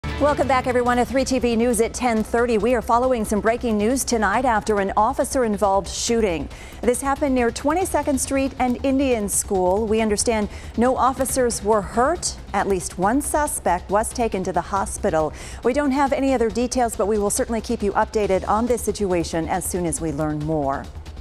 Suspect hurt after officer-involved shooting in central Phoenix
A suspect was taken to the hospital after an officer-involved shooting in central Phoenix.